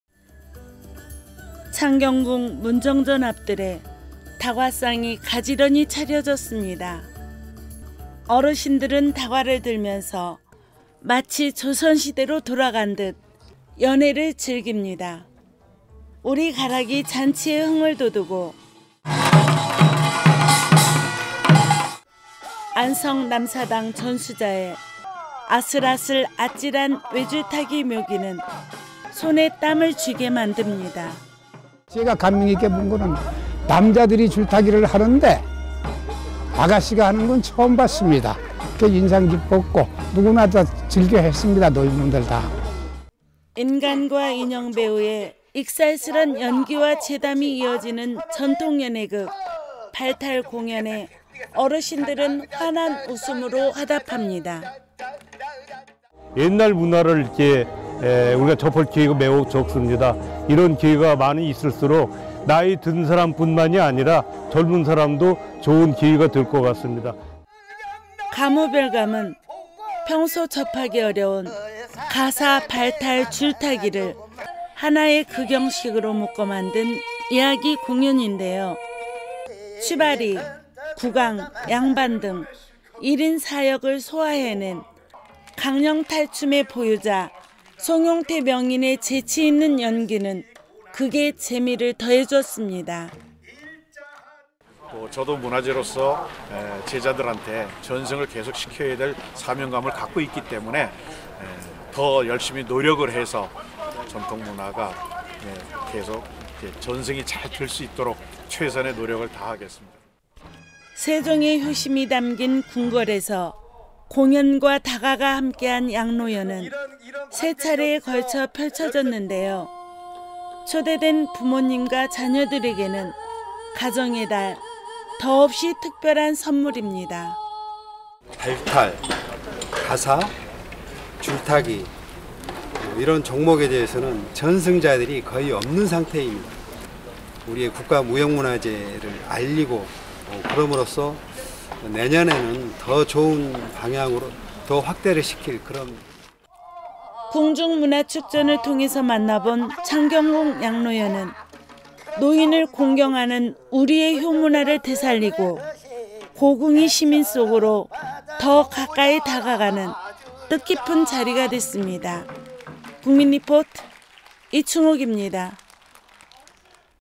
[국민리포트] 창경궁에서 열린 특별한 양로 잔치♥
예로부터 우리나라
왕실에서는
노인들을 위해
연회를 베푸는
양로연을 중요한
국가 행사의
하나로 여겼는데요.

가정의 달
5월을 맞아
효의 의미를
되새기는 양로
잔치가 창경궁에서
펼쳐졌습니다.